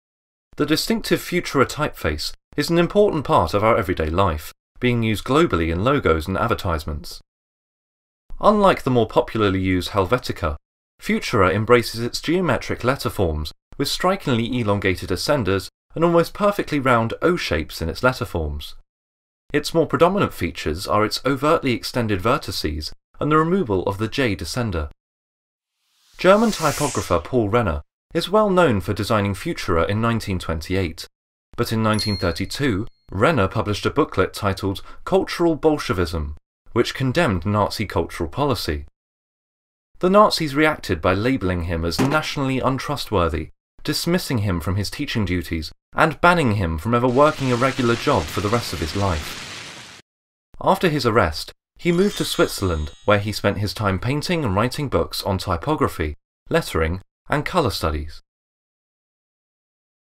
Paul Renner: The Creation of Futura
A one-minute animation that explains the life of Paul Renner and his creation of the typeface Futura in 1928.